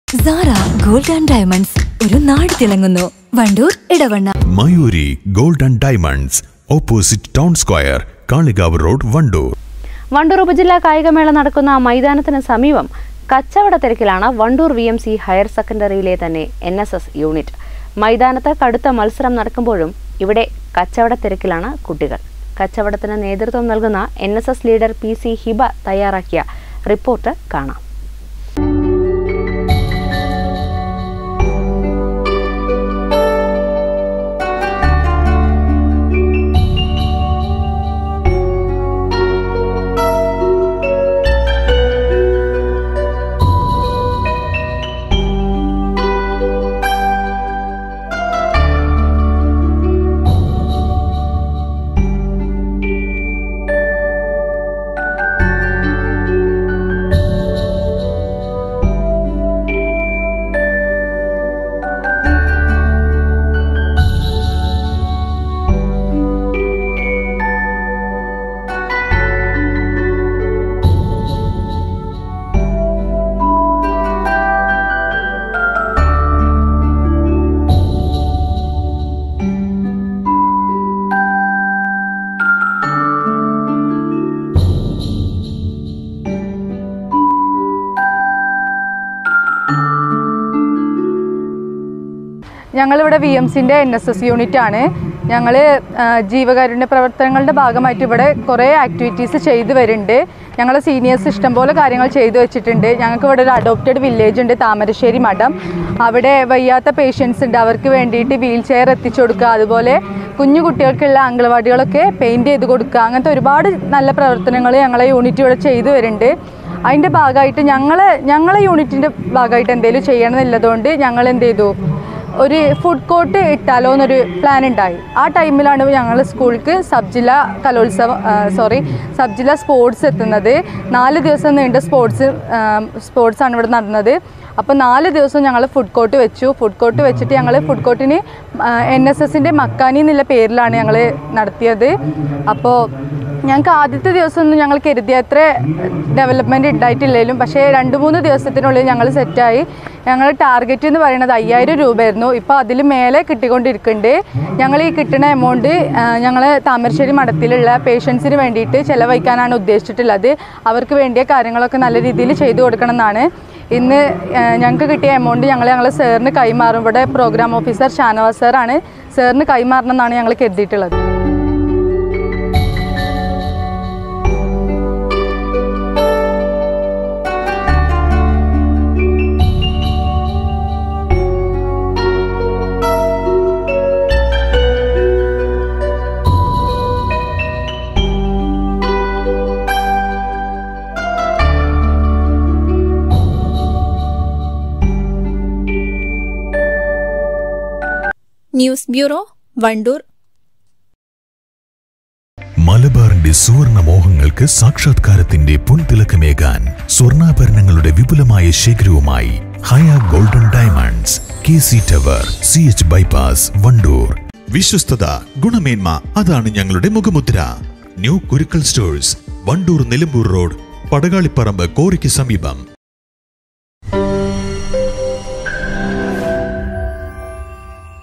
മൈതാനത്ത് കടുത്ത മത്സരം നടക്കുമ്പോഴും കച്ചവടത്തിരക്കിലാണ് ,NSS യൂണിറ്റ്
NSS ലീഡർ PC ഹിബ തയ്യാറാക്കിയ റിപ്പോർട്ട് കാണാം